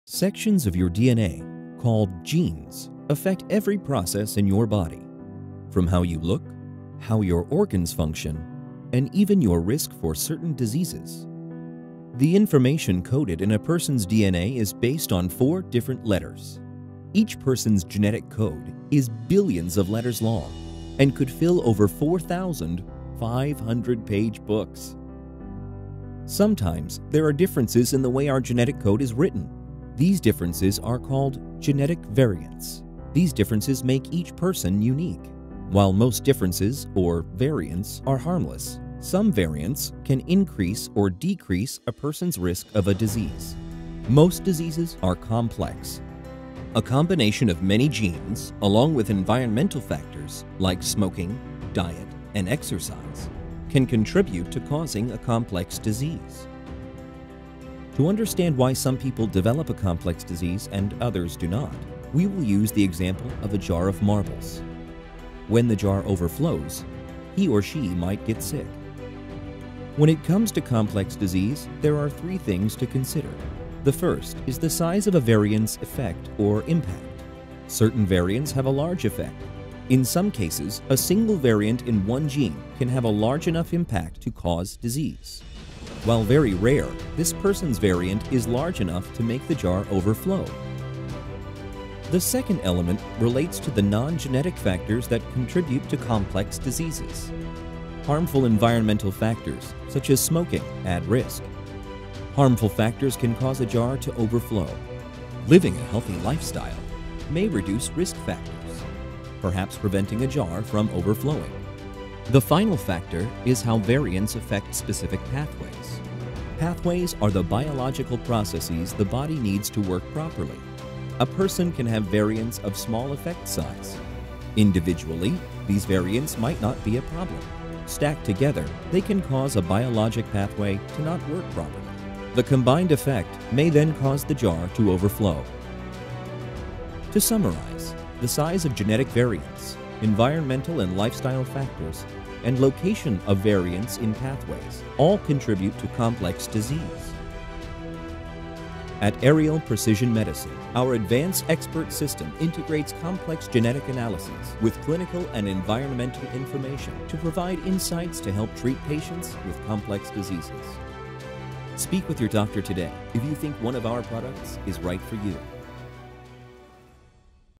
Intro to Complex Genetics - Ariel Precision Medicine
Our genes affect every process in our bodies, from how we look, to how our organs function, and even our risk for certain diseases. Most diseases are complex. The combination of many genes along with lifestyle factors like smoking, diet, and certain medications can contribute to causing a complex disease.  It can be difficult to determine the actual cause for a complex disease because there are so many risk factors to consider.
This is why Ariel created a new precision medicine A-I-powered platform that is transforming genetic and clinical data into new insights for the diagnosis and management of complex health conditions like pancreatitis and hypertriglyceridemia. Ariel integrates complicated genetic data with clinical and environmental information to provide deeper insights into the underlying cause(s) of complex diseases to enable more targeted and personalized clinical care.

Speak to your doctor today to see if one of our products is right for you!